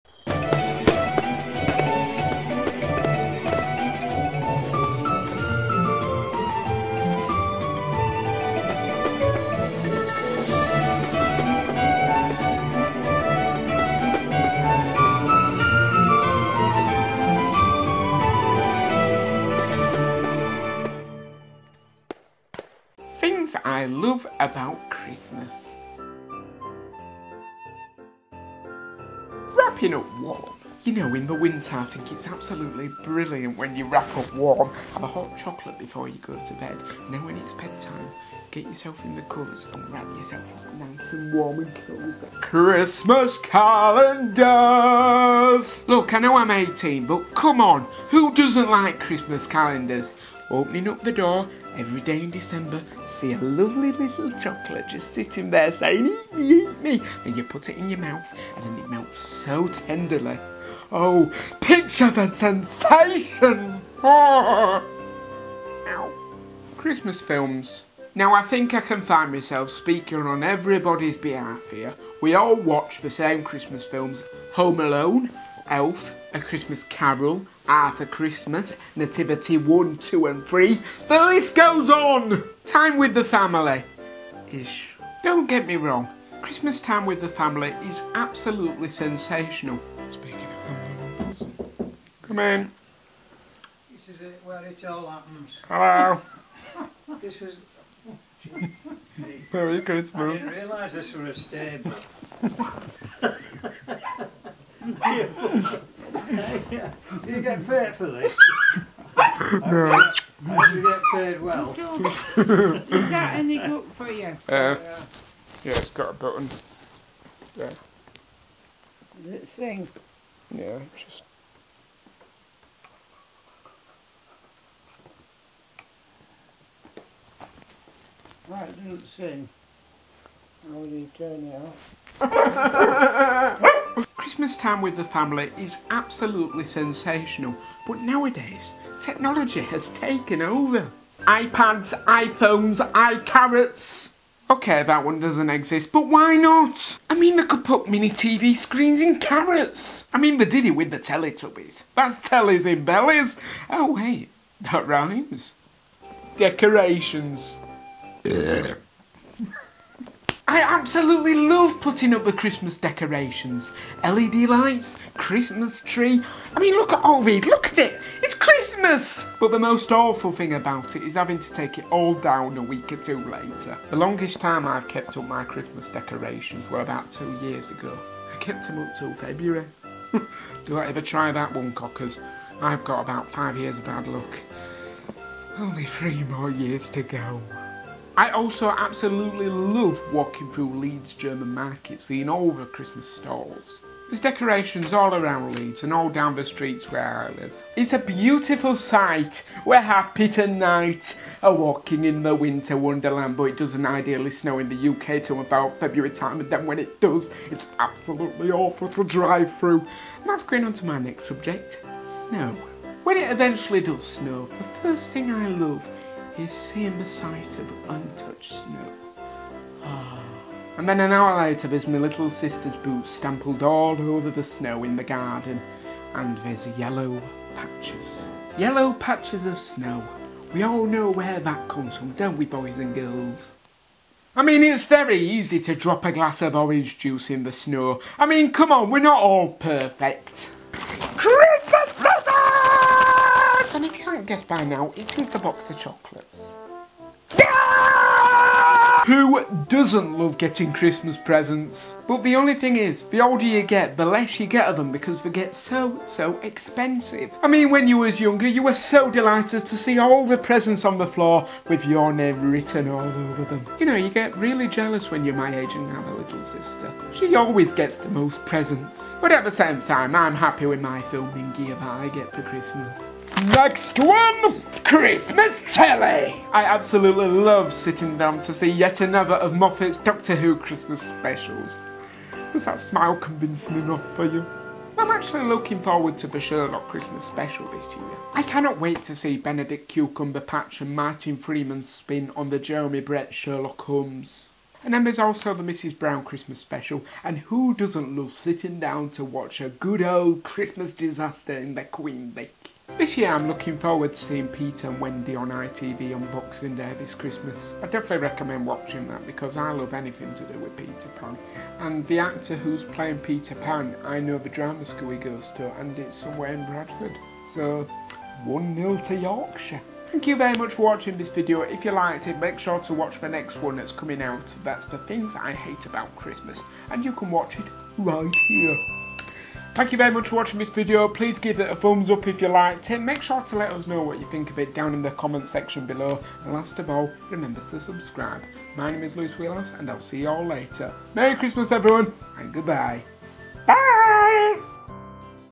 Things I Love About Christmas!
Lewis' Vlog #6 - Things I Love About Christmas

Hello there everyone, my name is Lewis Wheelhouse and in this video I am going to be listing the 'Things I Love About Christmas'.

I'd like to know what Things You Love About Christmas, Let me know down in the comments below!

If you enjoyed this video, Do give it a big THUMBS UP!